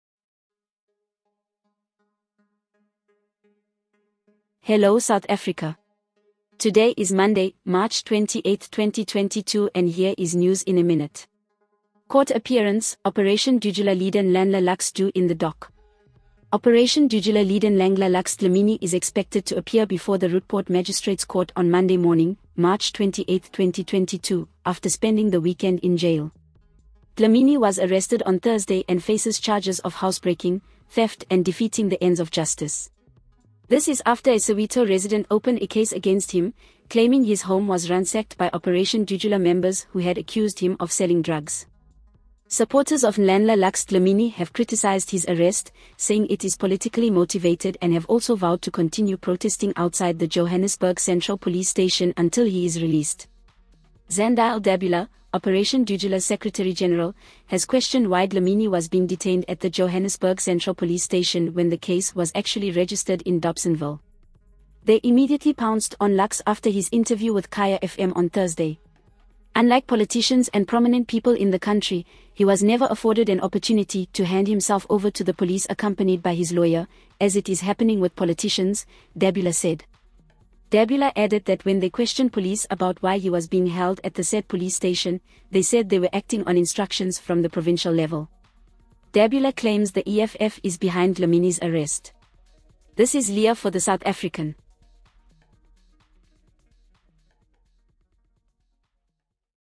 NEWS IN A MINUTE: Court appearance 'Operation Dudula leader Nhlanhla Lux due in the dock'
Operation Dudula leader Nhlangla Lux Dlamini is expected to appear before the Roodepoort Magistrate’s Court on Monday morning, 28 March 2022, after spending the weekend in jail

Videos: Twitter/newslivesa